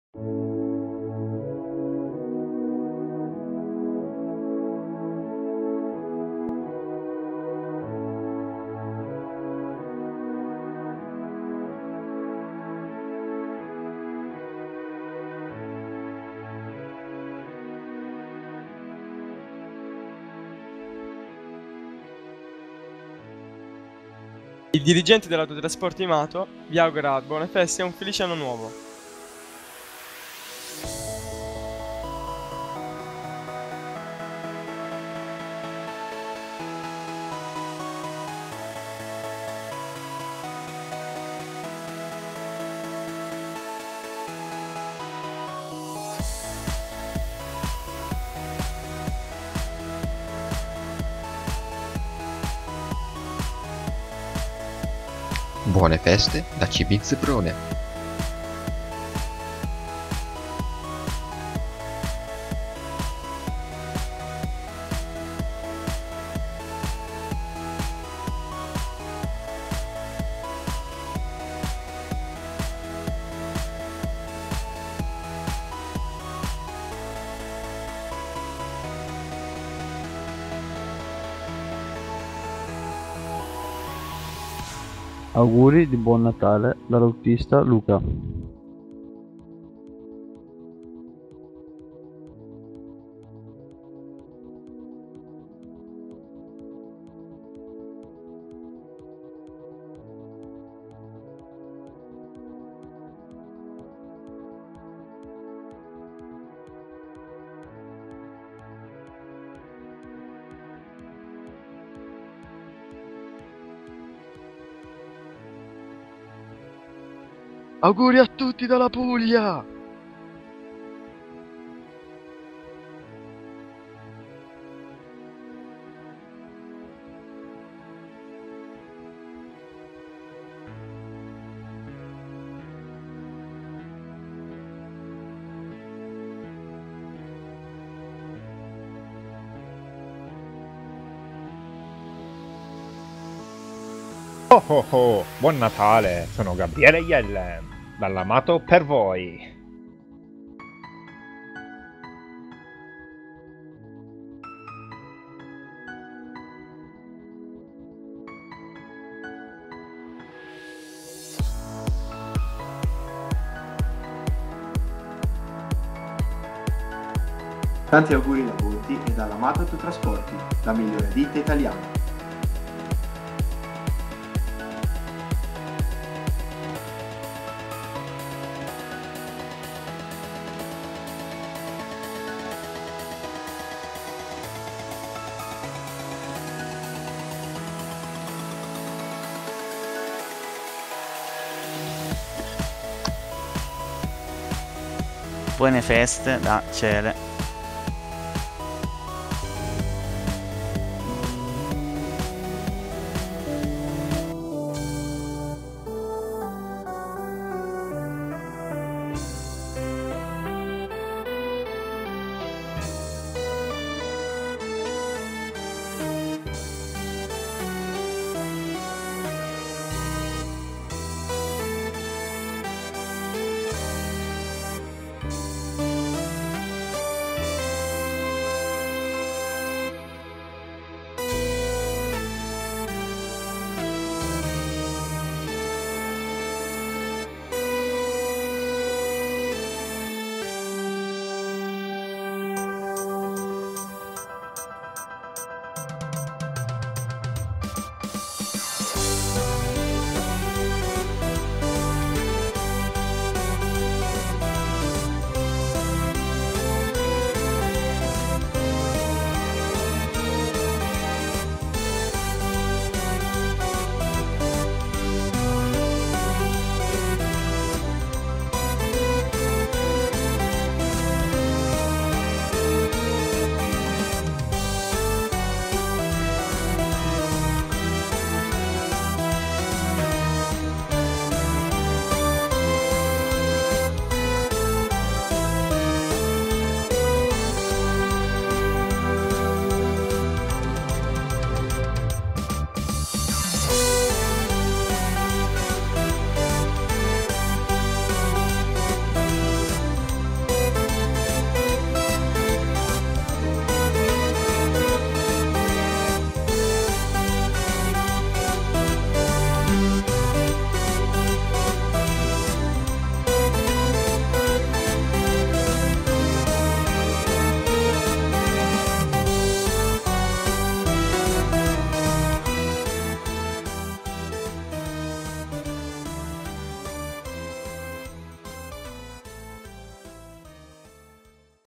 Autotrasporti MATO - Buone Feste!
I nostri più sentiti auguri di un Buon Natale e un Felice Anno Nuovo a tutti i nostri fans.
Video realizzato con la collaborazione dei dipendenti (e delle pseudo segretarie)
Musiche realizzate da: TheMirko96
BUONE FESTE!